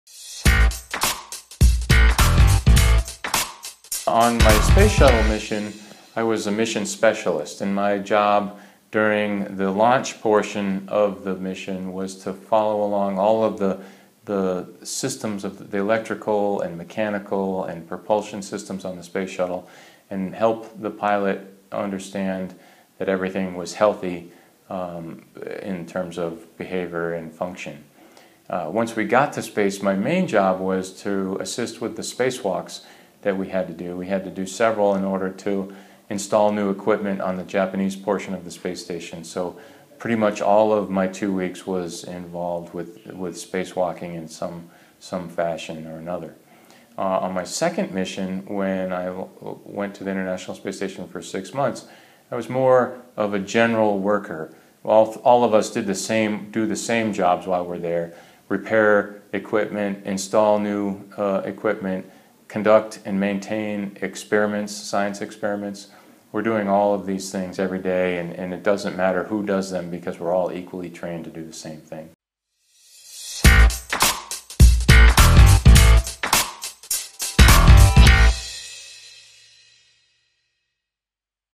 Como é a rotina de um astronauta
Chris Cassidy, atualmente o astronauta mais importante da NASA responde as perguntas do Na Prática sobre a sua trajetória.